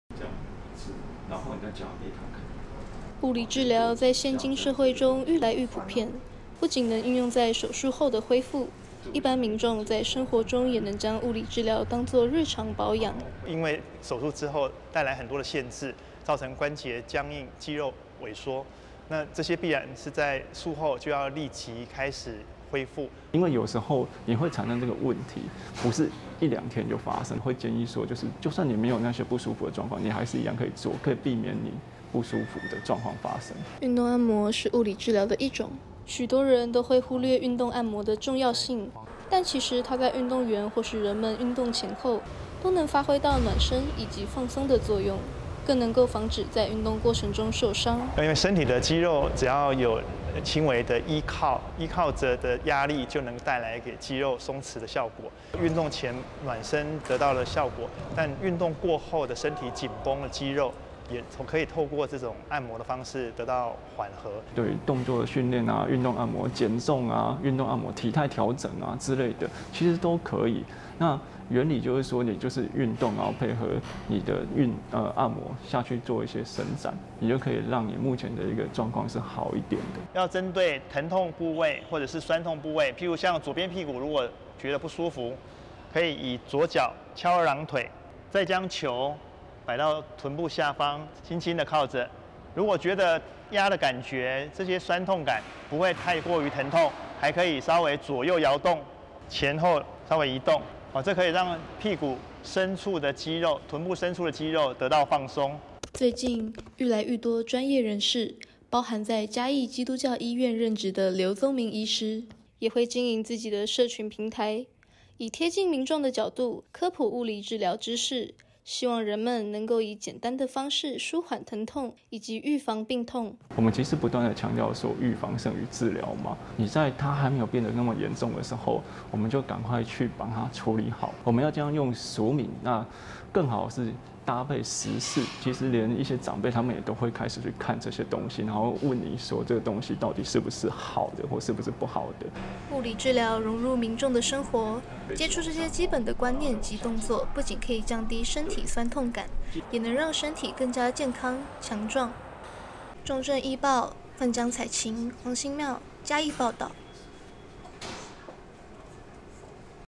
「扭」轉痠痛 物治與你息息相關
【記者　黃伈妙、范姜采晴／嘉義報導】

物理治療不僅能運用在術後恢復，在生活中物理治療也能當作日常保養，包括運動員在運動前後能夠以運動按摩放鬆肌肉、預防傷害。在現代社會中，工作、學習常常需要久坐，也十分容易造成身體部位痠痛，物理治療也能舒緩人們的不適。